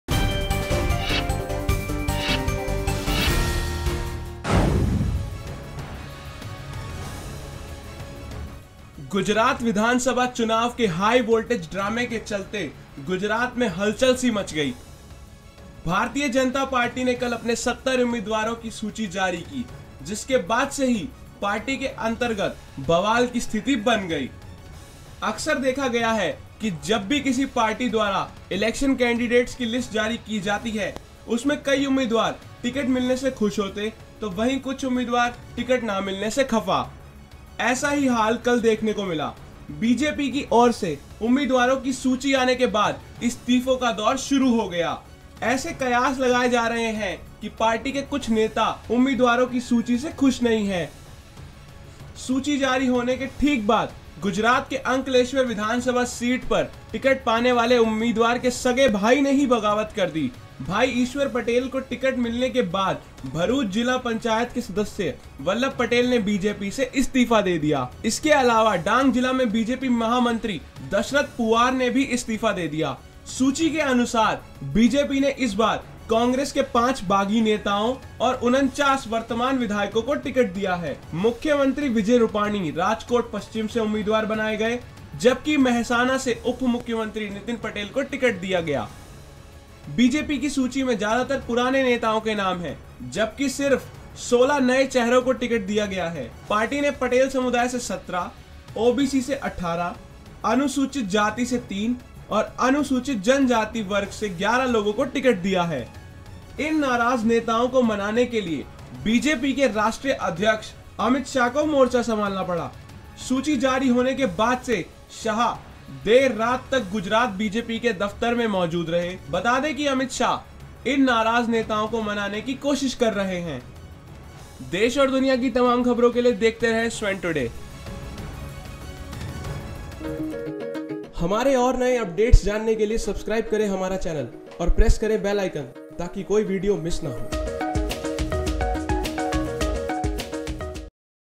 गुजरात चुनाव: बीजेपी में बगावत ! Gujarat Election
गुजरात विधानसभा चुनाव के हाई वोल्टेज ड्रामे के चलते गुजरात में हलचल मच गयी ,
भारतीय जनता पार्टी ने कल अपने 70 उमीदवारो की सूचि जारी की ,जिसके बाद से ही पार्टी के अंतर्गत बवाल की स्तिथति बन गयी हे,
अक्सर देखा गया हे की जब भी किसी पार्टी द्वारा इलेक्शन कैंडिडेट्स के लिस्ट जारी की जाती हे, उसमे कई उमीदवार टिकट मिलने से खुश होते तो वही कुछ उमीदवार टिकट न मिलने से खफा , ऐसा ही हाल कल dekhne ko mila बीजेपी की और से उमीदवारो की सूचि आने के बाद इस्तीफो का दौर शुरू हो गया ,
ऐसे कयास लगाए जा रहे हे की पार्टी के कुछ नेता उमीदवारो की सूचि से खुश नहीं हे, सूची जारी होने के ठीक बाद गुजरात के अंकलेश्वर विधानसभा सीट पर टिकट पाने वाले उम्मीदवार के सगे भाई ने ही बगावत कर दी. भाई ईश्वर पटेल को टिकट मिलने के बाद भरुच जिला पंचायत के सदस्य वल्लभ पटेल ने बीजेपी से इस्तीफा दे दिया.
इसके अलावा डांग में जिला बीजेपी महामंत्री दशरथ पुवार ने भी इस्तीफा दे दिया.
सूचि के अनुसार बीजेपी ने इस बार कांग्रेस के पांच बागी नेताओं और 49 वर्तमान विधायकों को टिकट दिया हे , मुख्यमंत्री विजय रूपाणी राजकोट पश्चिम से उम्मीदवार बनाए गए हैं, जबकि महेसाणा से उपमुख्यमंत्री नितिन पटेल को टिकट दिया गया है,
बीजेपी की सूचि में ज्यादातर पुराने नेताओ के नाम हे , जबकि सिर्फ 16 नए चेहरों को टिकट दिया गया है. पार्टी ने पटेल समुदाय से 17, ओबीसी से 18, अनुसूचित जाति से 3 और अनुसूचित जनजाति वर्ग से 11 लोगों को टिकट दिया है.
इन नाराज़ नेताओ को मनाने के लिए बीजेपी के राष्ट्रीय अध्यक्ष अमित शाह को मोर्चा संभालना पड़ा. सूचि जारी होने के बाद से शाह देर रात तक गुजरात बीजेपी के दफ्तर में मौजूद रहे, बता दे की अमित शाह इन नाराज़ नेताओ को मानाने की कोशिश कर रहे हे ,
अब देखना ये होगा की पार्टी के अंतर्गत ही अगर बगावत के सुर जोर पकड़ेंगे तो पार्टी के सामने काफी मुश्किले आ सकती हे।